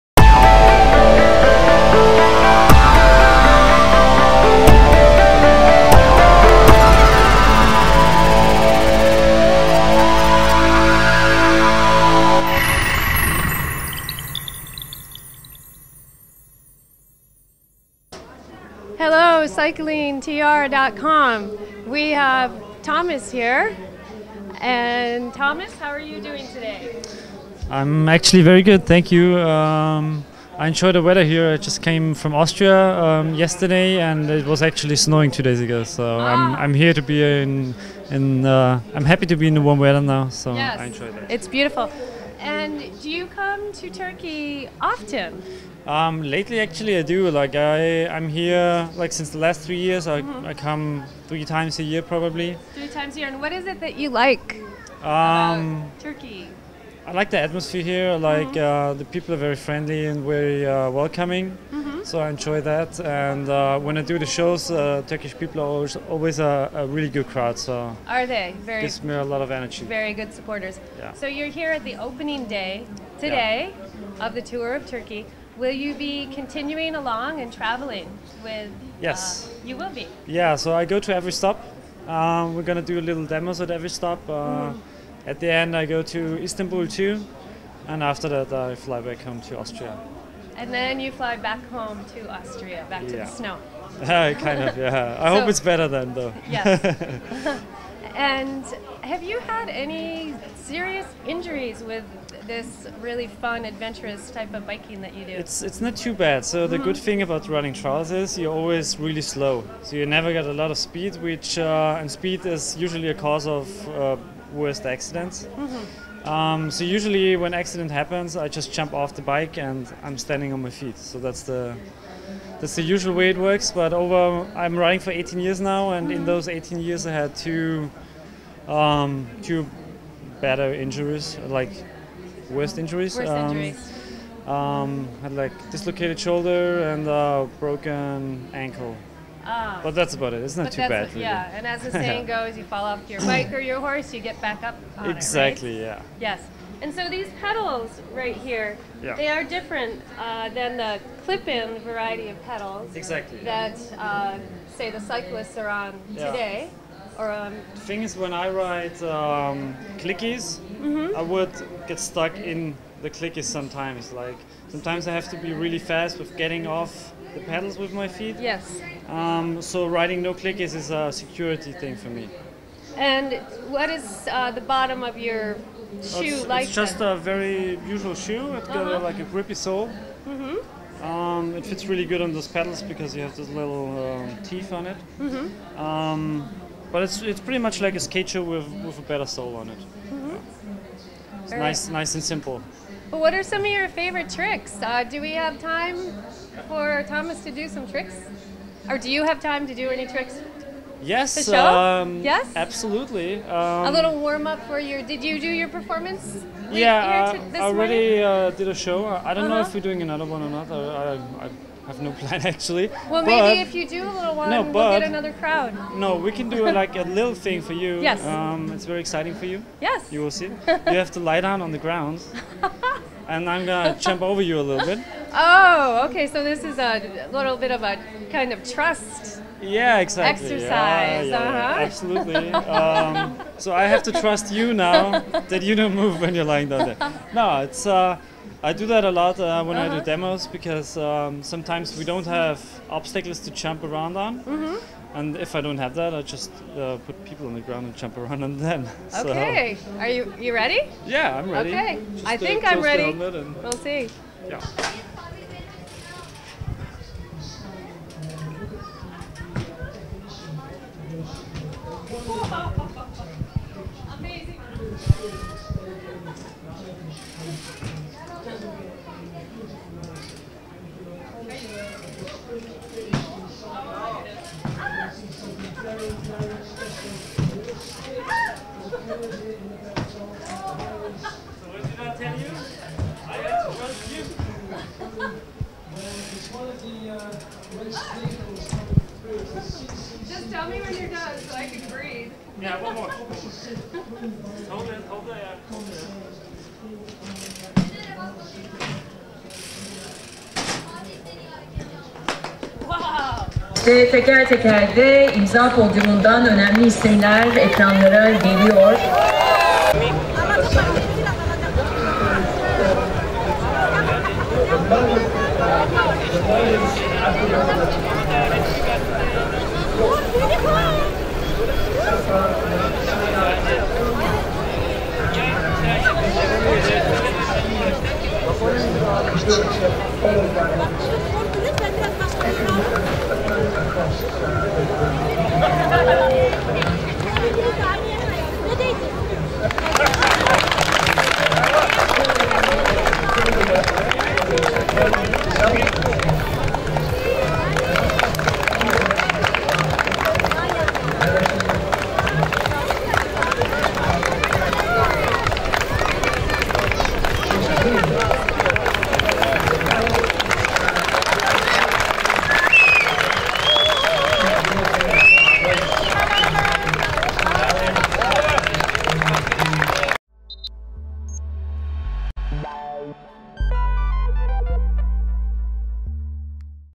2013 Tour Of Turkey - Thomas Oehler - Röportaj ve Performans
2013 Tour Of Turkey  Alanya etabında ünlü TrailBike performanscısı Thomas Oehler ile küçük bir röportaj yaptık...

Türkiyenin Bisiklet ve Triatlon Portalı